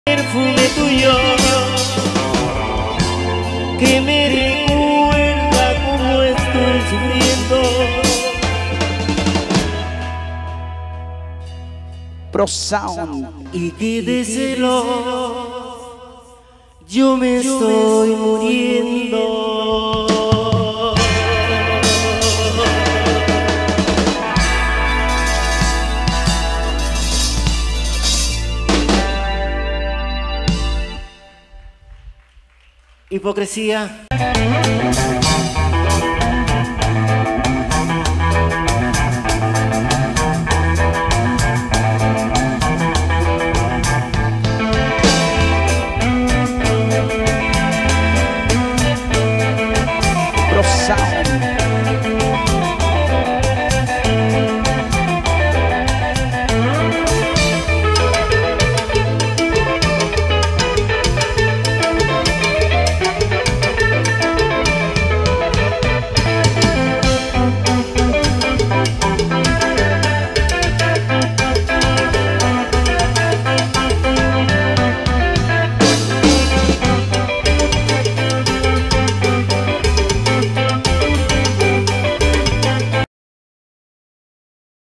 2014 los pasteles verdes en vivo
este video fue tomado con un telefono samsung. como test d video ..gravado en 24 canales. editado y mix en estudio prosound en...
richmond ca. renta d audio y gravacion para todo tipo de evento para todo el norte de california